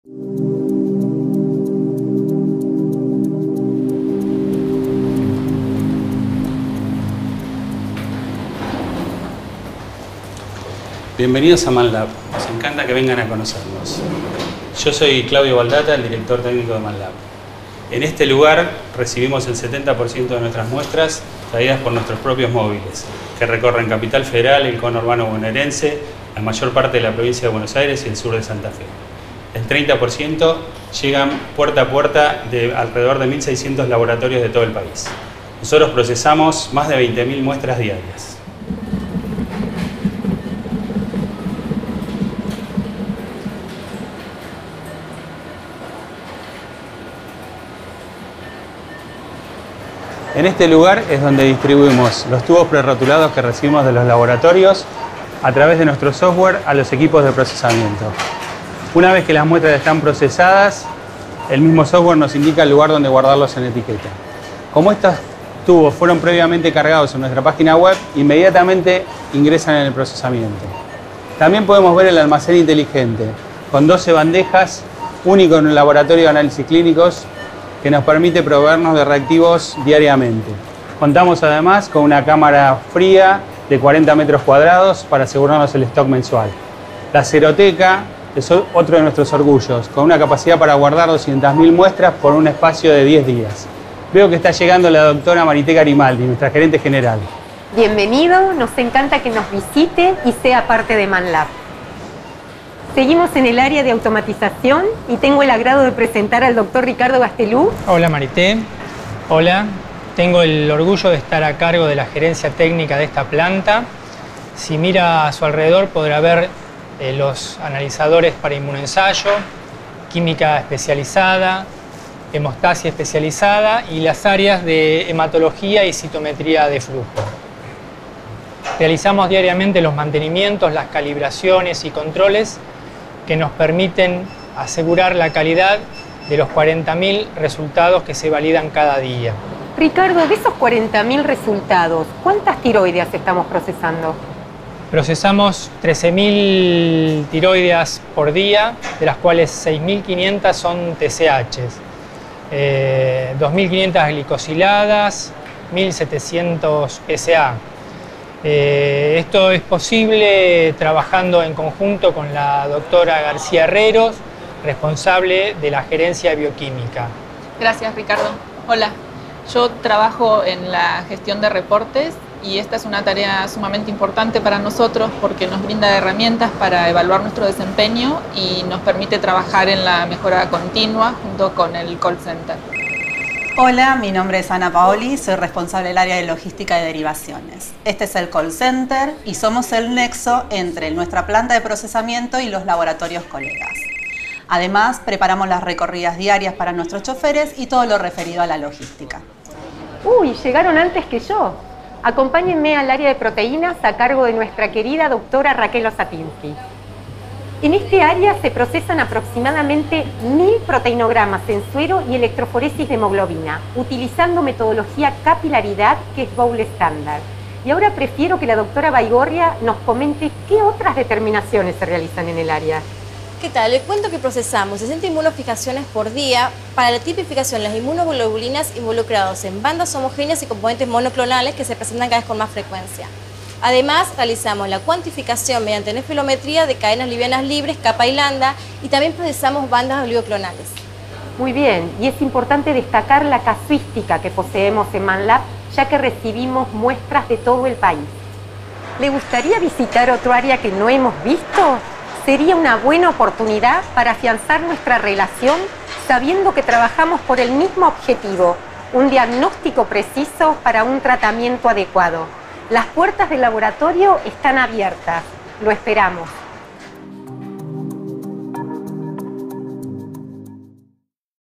Tour 360 por los laboratorios de MANLAB
Producción General: Matías F. Nielsen
Rodaje: Manuel Forbes y Realidad 360
Postproducción: Realidad 360